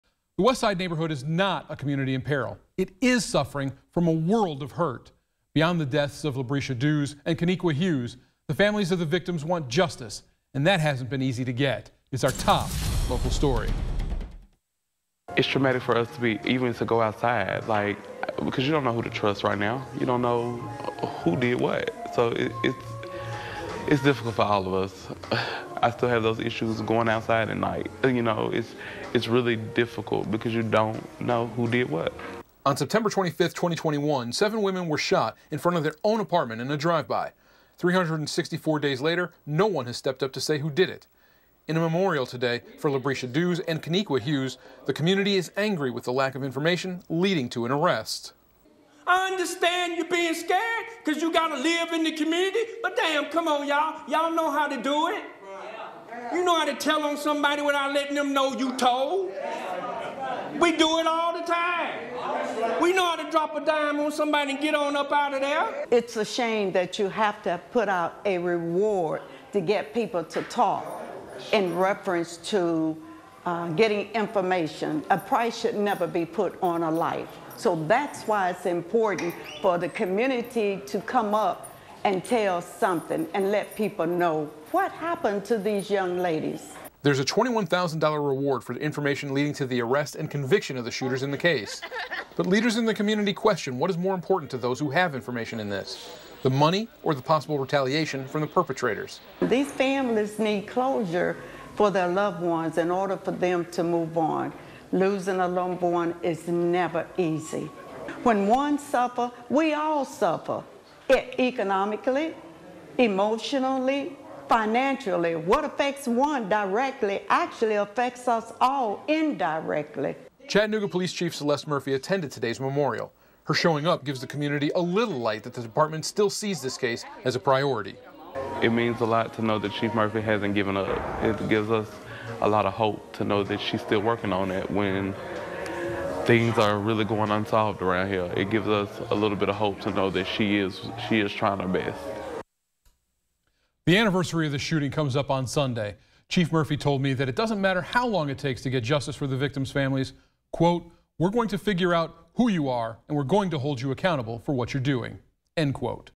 Memorial for the women killed at 2021 Westside Reunion becomes a call for information on the
The one-year anniversary of the still-unsolved shooting of seven women at a local block party is Sunday